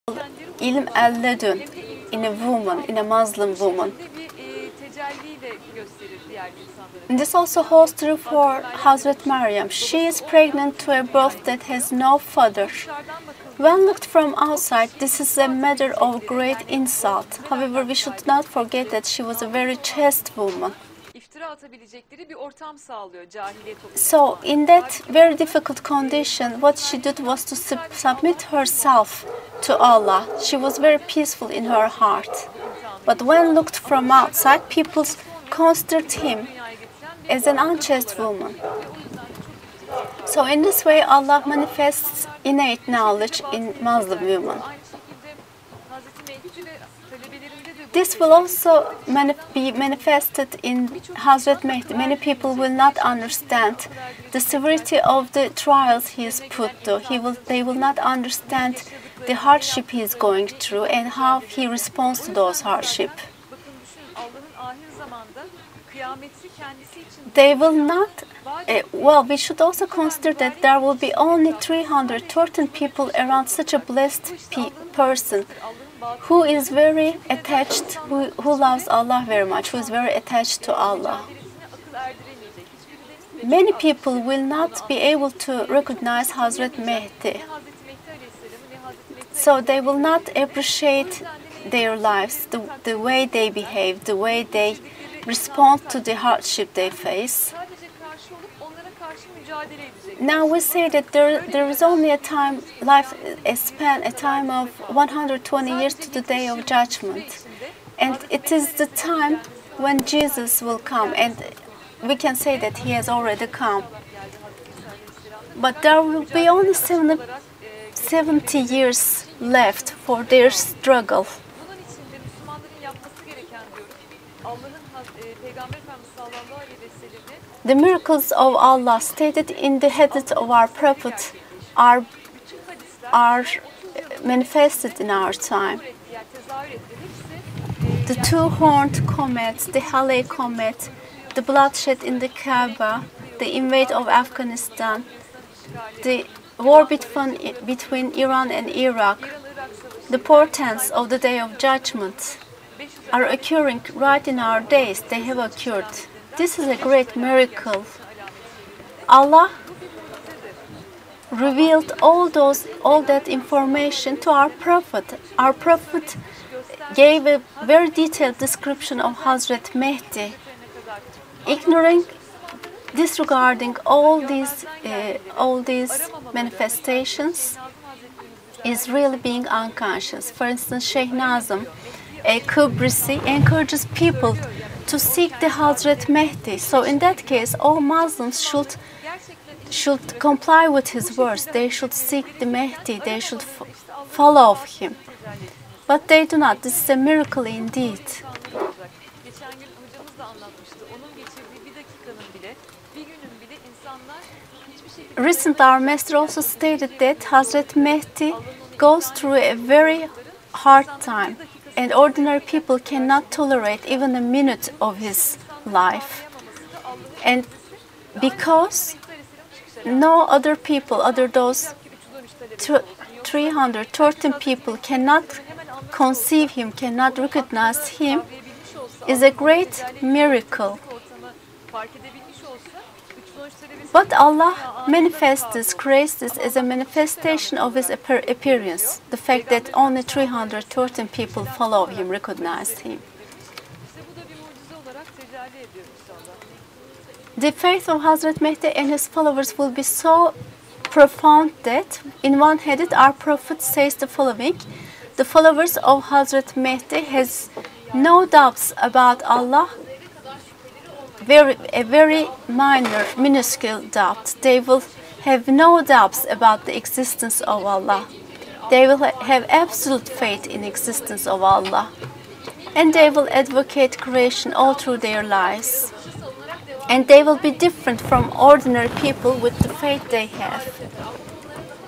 Hazrat Maryam is also an example to students of Mahdi
Adnan Oktar's live talk on A9 TV with simultaneous interpretation
You can watch live interviews of Adnan Oktar from A9 TV